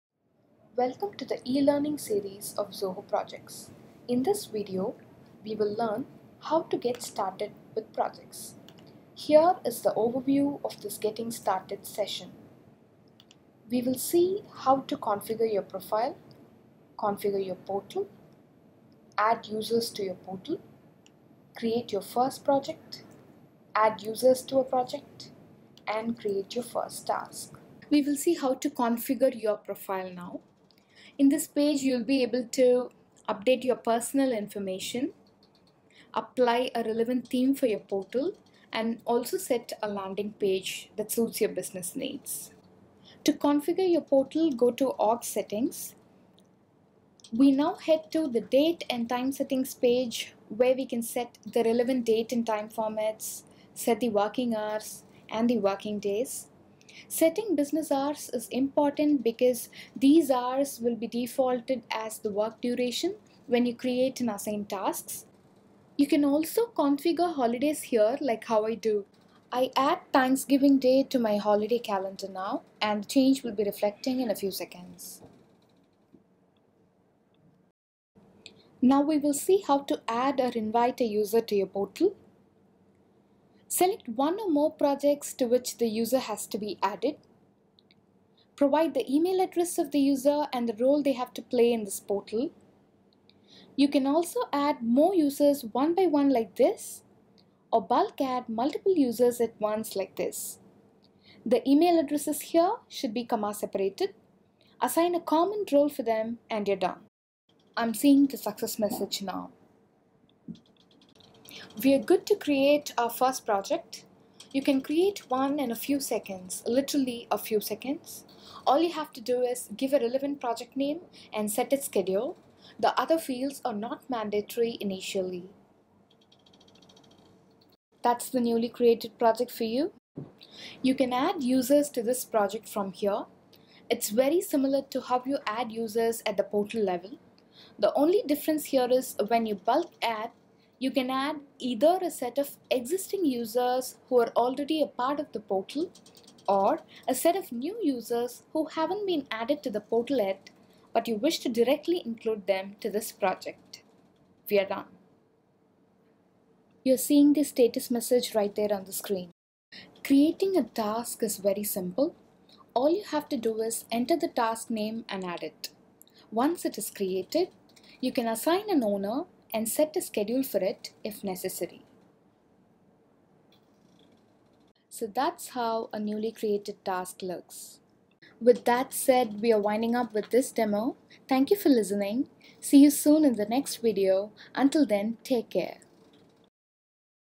Getting Started with Zoho Projects
This video covers the basics of how to get started with Zoho Projects.

0:28 Configure your profile

0:44 Configure your portal

1:19 Add user to the portal

1:50 Create a project

2:10 Add user to the project

2:39 Create a task